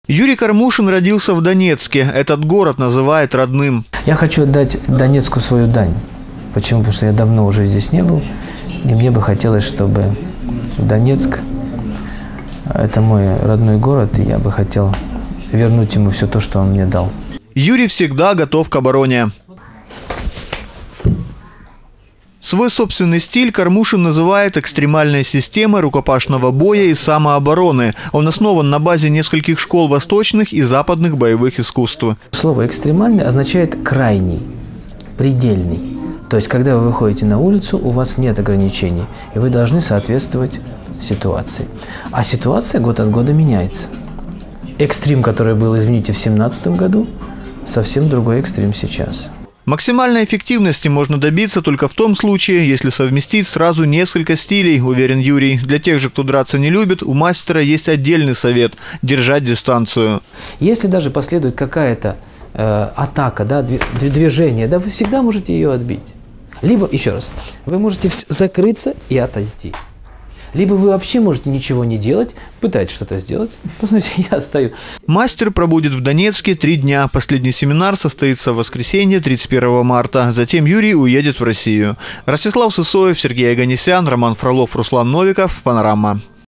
Эксклюзивный урок самообороны
В Донецк приехал Юрий Кормушин, актер, мастер боевых искусств и ведущий программы «Школа выживания» на федеральном канале России. В столице Донбасса он проводит семинары по рукопашному бою. Их будет три. Эксклюзивный урок самообороны мастер провел и для  журналистов телеканала «Юнион».